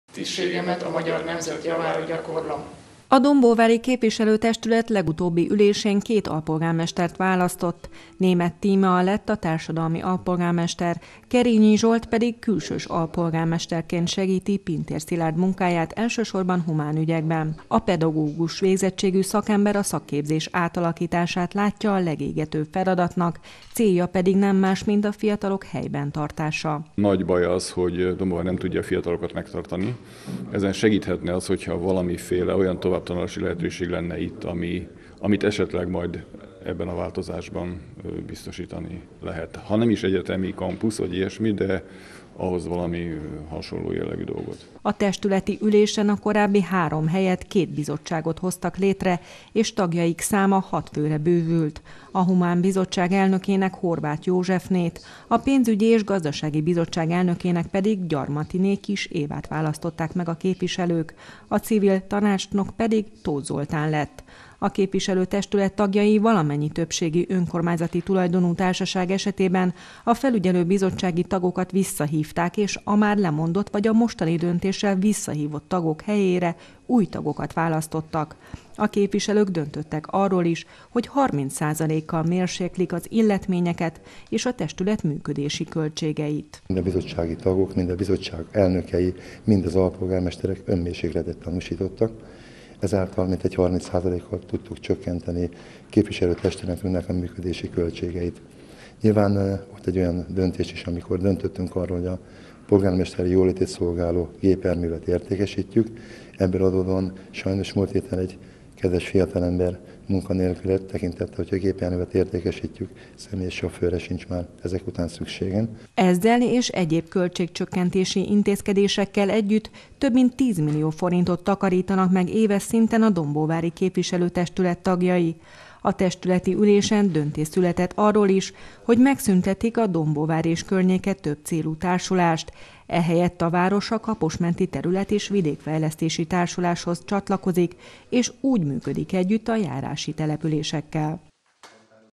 30 százalékkal csökken a dombóvári képviselő-testület működési költségei 20191031
Béreket csökkentettek, felügyelő bizottsági tagokat hívtak vissza és költségcsökkentéssel kapcsolatos döntéseket hoztak a dombóvári képviselő testület tagjai csütörtöki ülésükön.